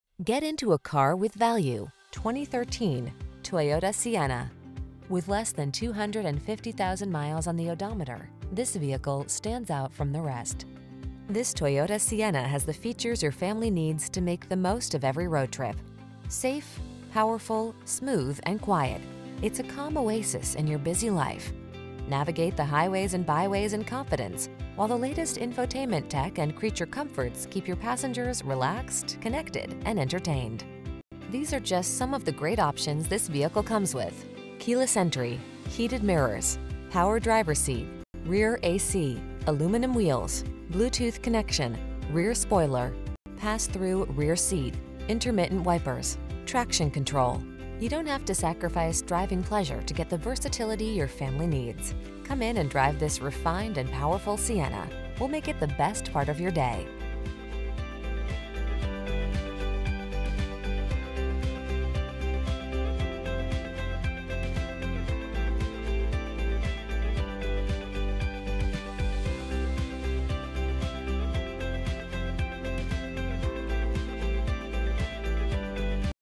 2013 Toyota Sienna LE UT Orem, Provo, Pleasant Grove, Vineyard, Lehi
Pre-Owned 2013 Toyota Sienna LE available at Brent Brown Toyota located in 1400 Sandhill Rd, Orem, UT, 84058. Servicing the Orem, Provo, Pleasant Grove, Vineyard, Lehi area.
Please mention the stock T62780B when you contact the dealership for this vehicle.